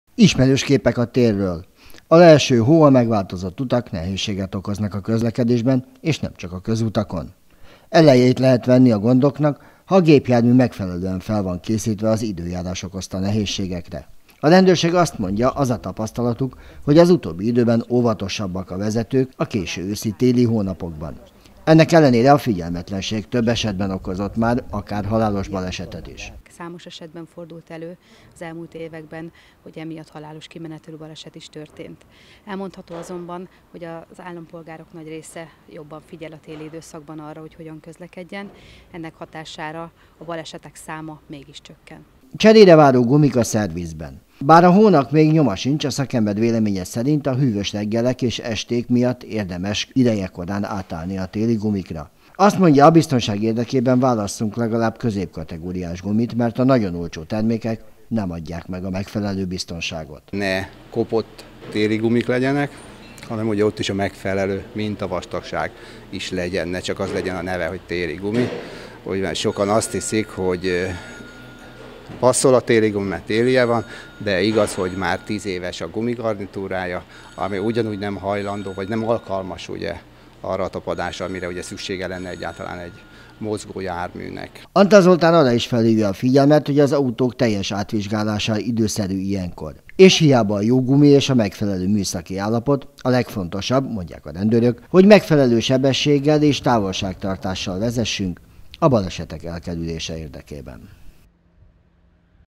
Fokozott balesetveszélynek teszi ki magát az, aki nem gondoskodik idejében a járműve téliesítéséről.
Az időjárás előrejelzés folyamatos lehülést jósol és bár nap közben még kellemes az idő, közlekedési szempontból a téli átállás idejét éljük. A rendőrség és a szakértők is állítják, fokozott balesetveszélynek teszi ki magát az, aki nem gondoskodik idejében a járműve téliesítéséről.